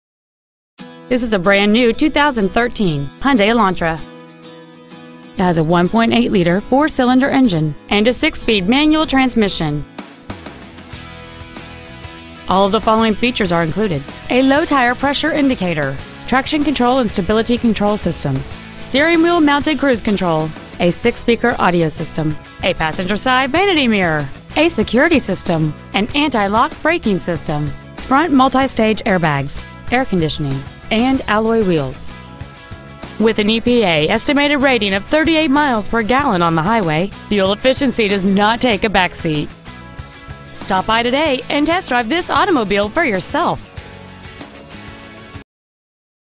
2013 Hyundai Elantra Miami FL
Year : 2013
 Make : Hyundai
 Model : Elantra
 Engine : 1.8l 4-cylinder dohc 16v dual cvvt
 Trans . : 6-speed manual
 Exterior : red
 Miles : 14
 Stock : E769100
 Interior : red

 Lehman Mazda